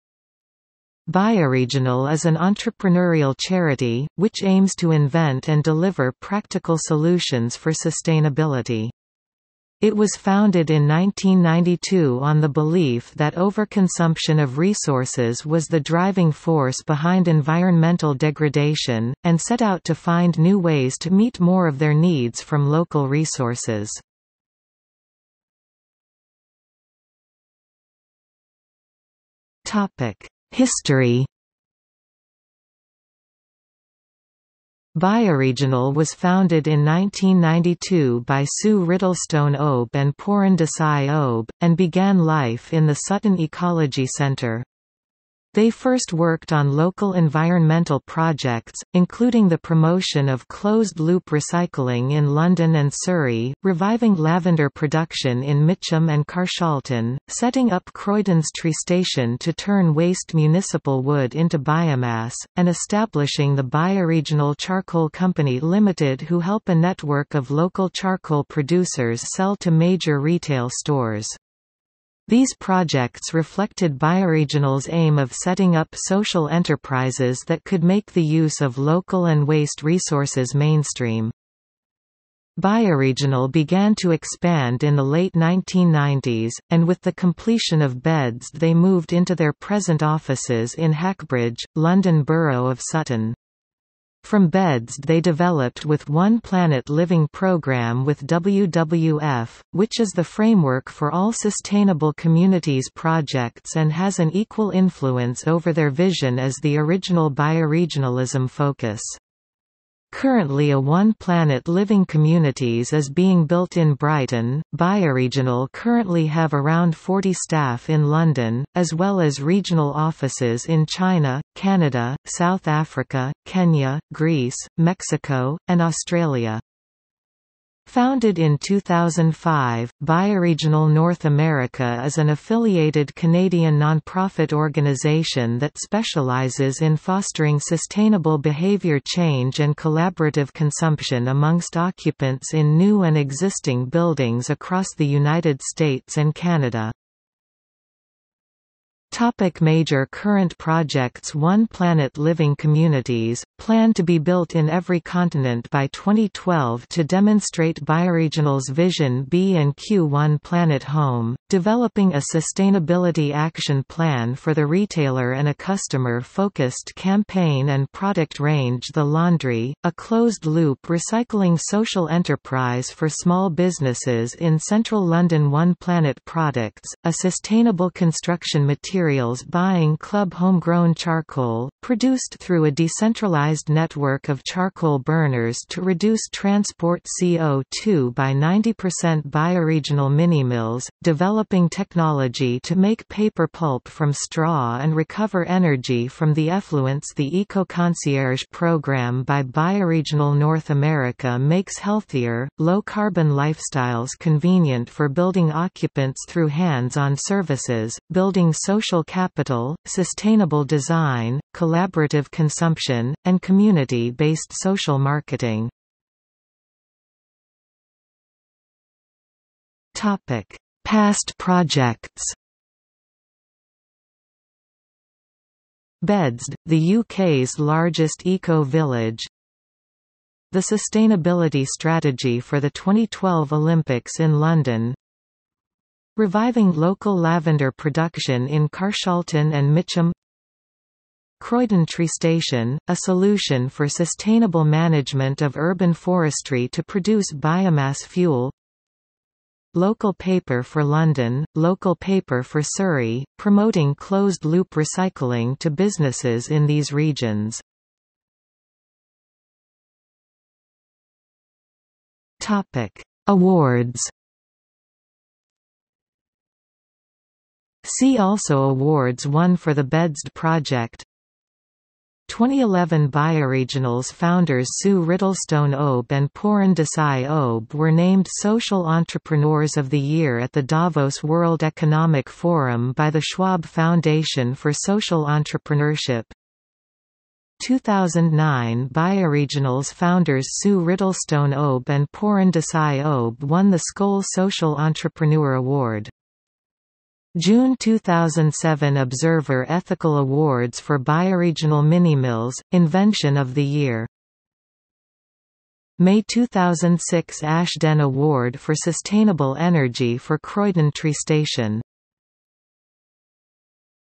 BioRegional | Wikipedia audio article
00:00:26 1 History
00:02:28 2 Major current projects
00:03:36 3 Past projects
00:04:18 4 Awards

- Socrates

SUMMARY
Bioregional is an entrepreneurial charity, which aims to invent and deliver practical solutions for sustainability. It was founded in 1992 on the belief that overconsumption of resources was the driving force behind environmental degradation, and set out to find new ways to meet more of their needs from local resources.